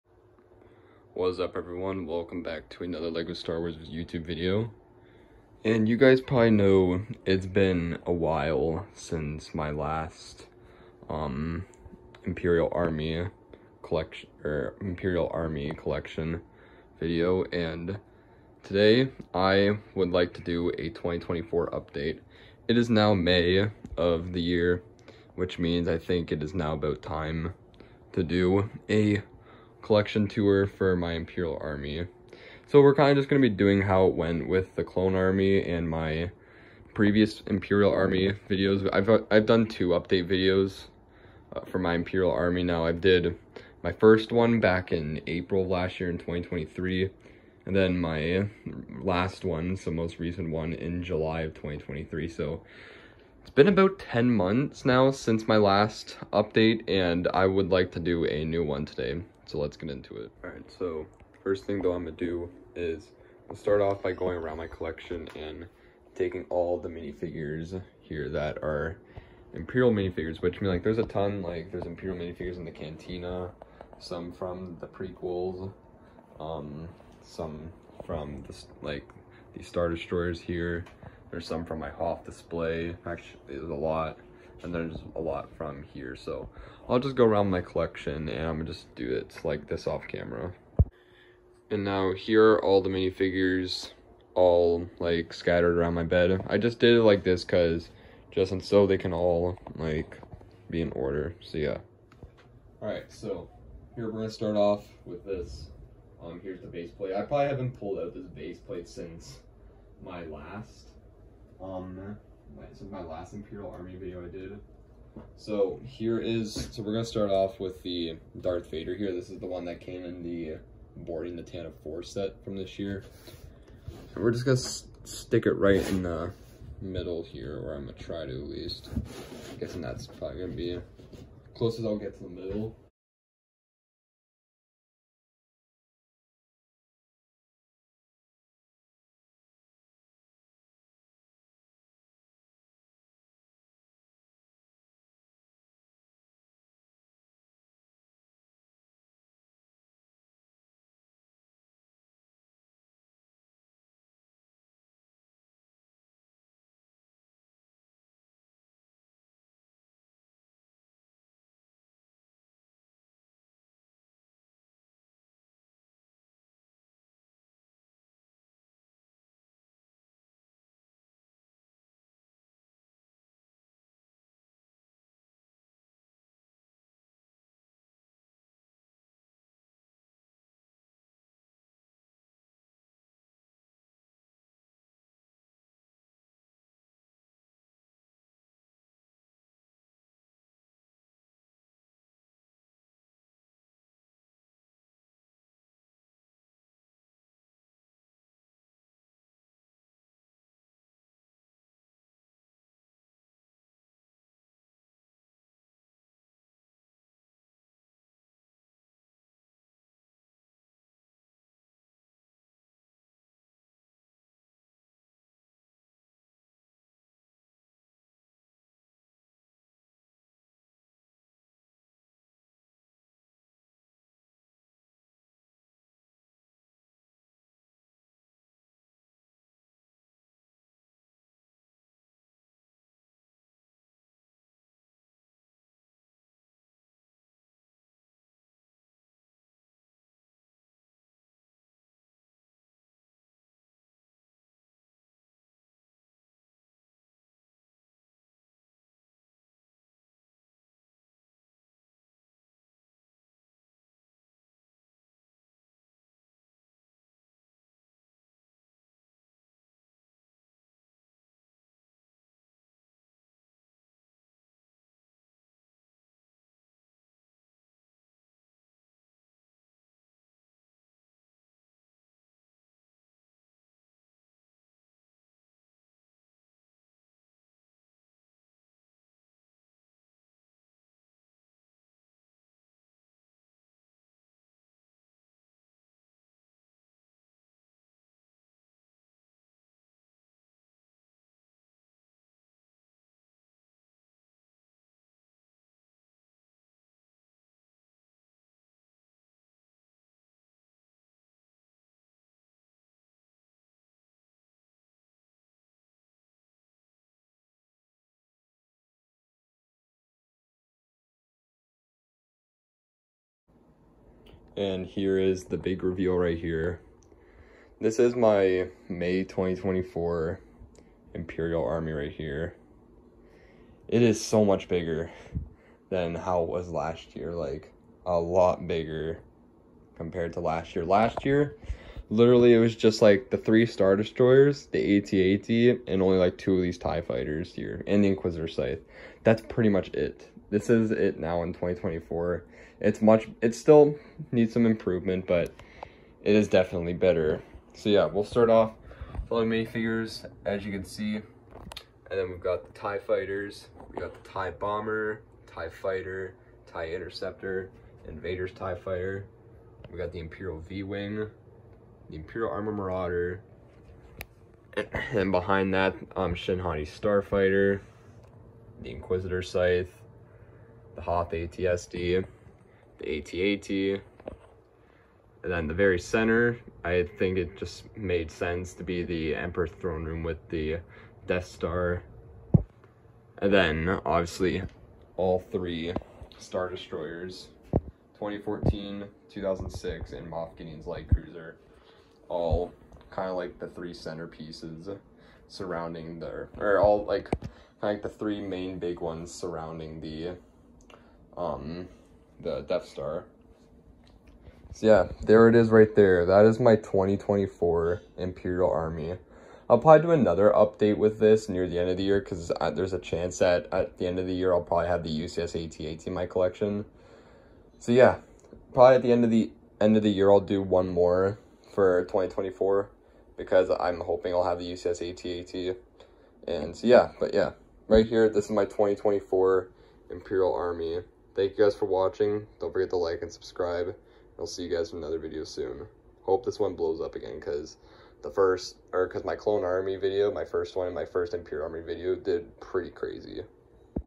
My Lego Star Wars Imperial Army!!! (2024 update)
My last update on this was in July of 2023 and I would say 10 months later it looks a hole lot bigger now…